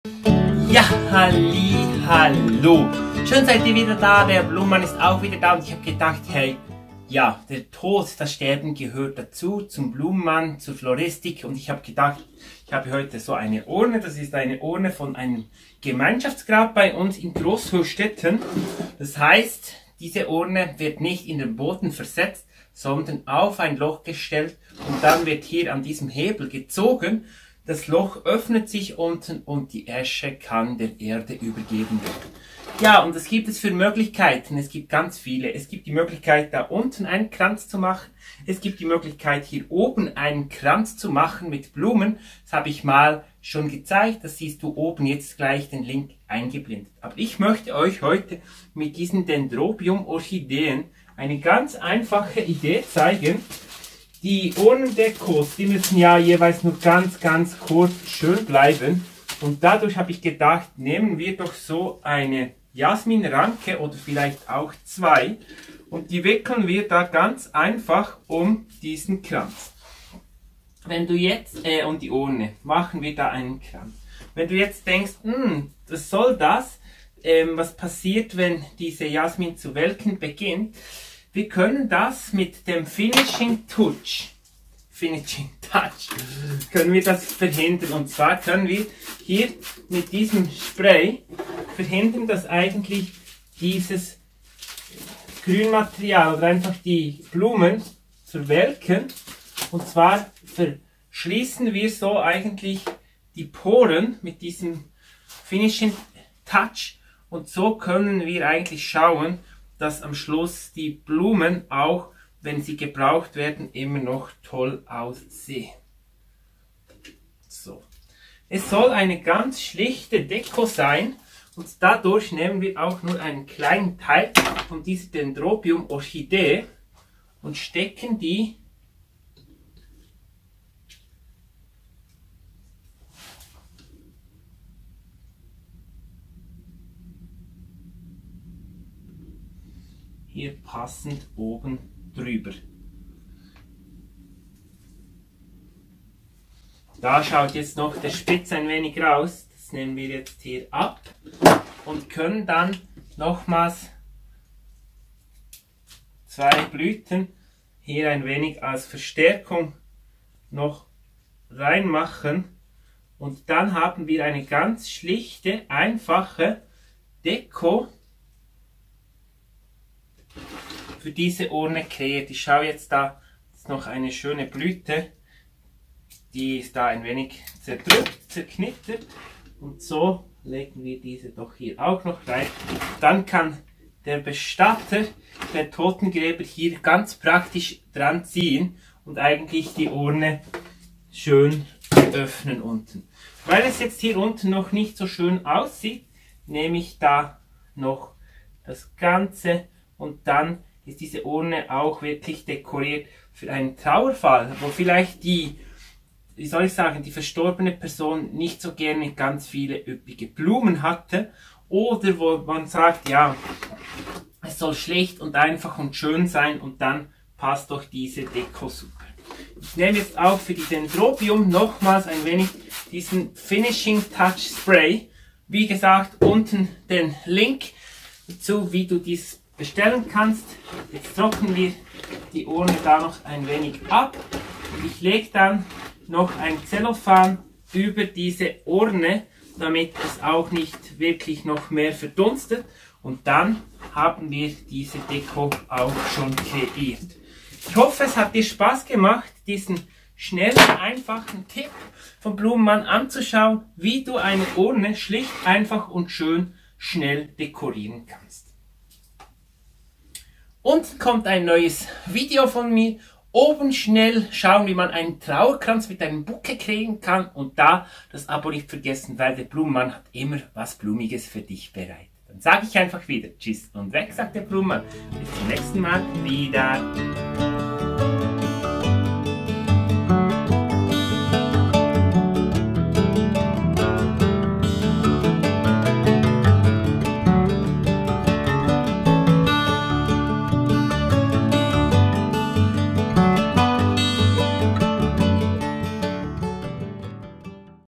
Urnen Deko selber machen - Dendrobium pink - Einfacher Urnenschmuck für Beerdigung
Der Blumenmann erklärt dir, wie du schnell und einfach eine eindrückliche Urnendekoration kreieren kannst. Lasse dich vom Blumenmann anleiten und inspirieren wie du kostengünstig und schnell eine Urne schmücken kannst.
Wie das passende Herz mit roten Rosen und Dendrobieum Orchideen Blüten kannst du hier sehen

Floralife Finishing Touch kaufen